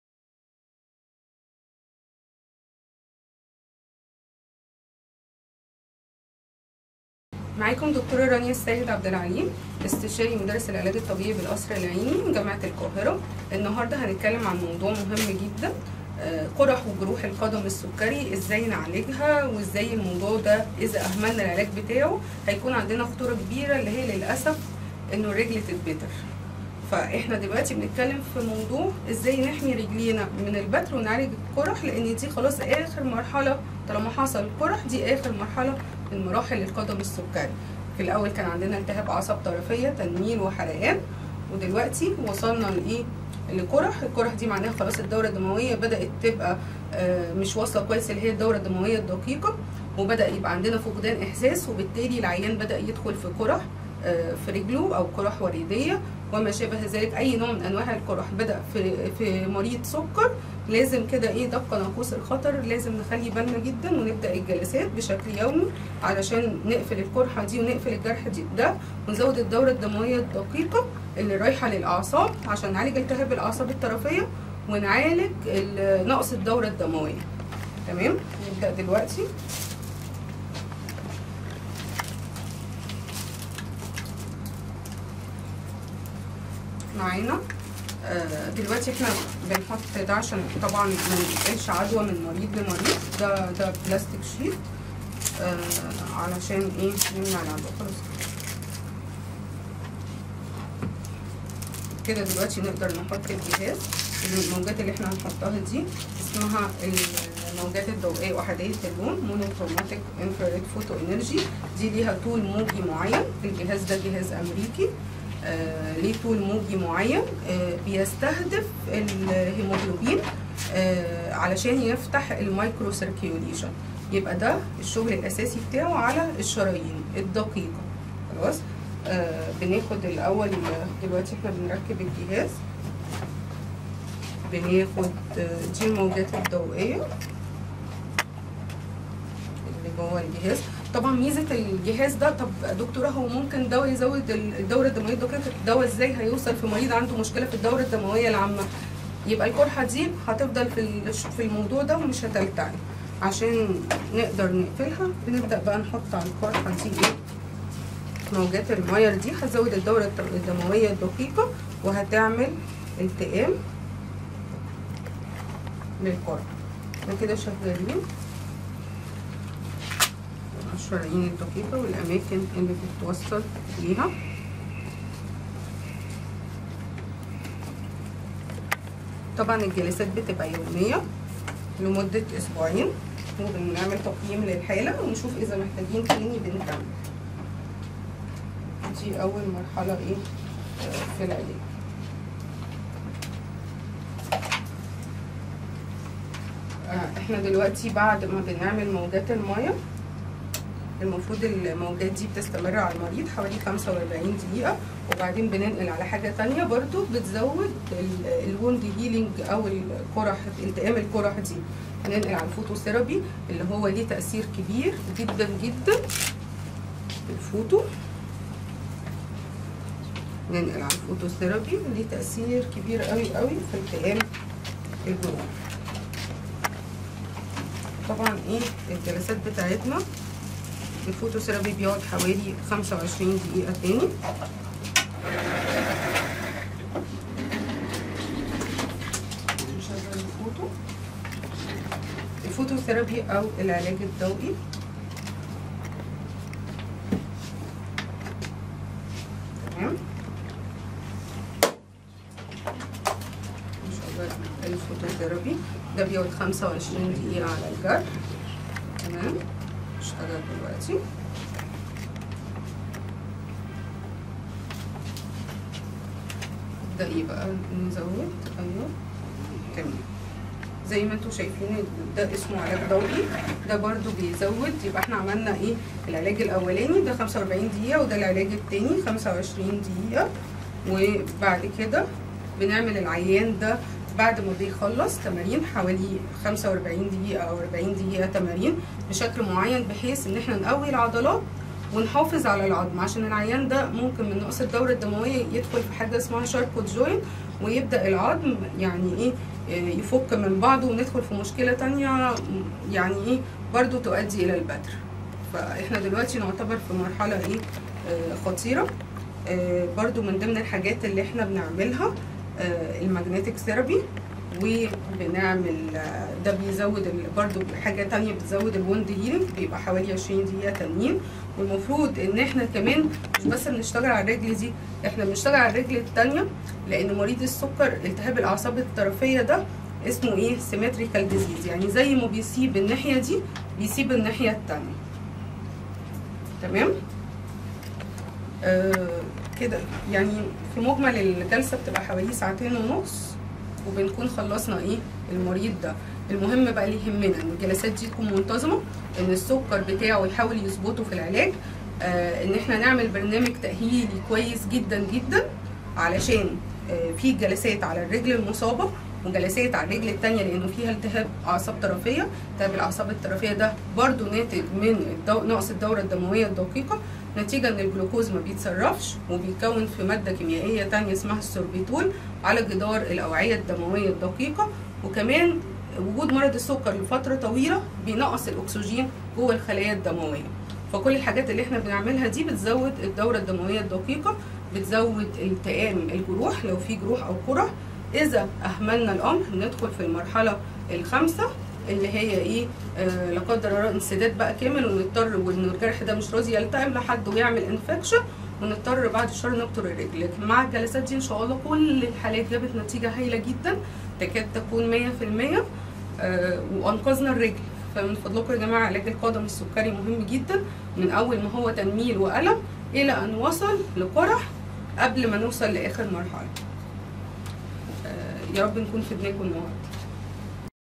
طريقة علاج القدم السكري والوقاية من البتر مع الدكتورة / رانيا السيد عبد العليم
عيادة المهندسين : 203 ش السودان - المهندسين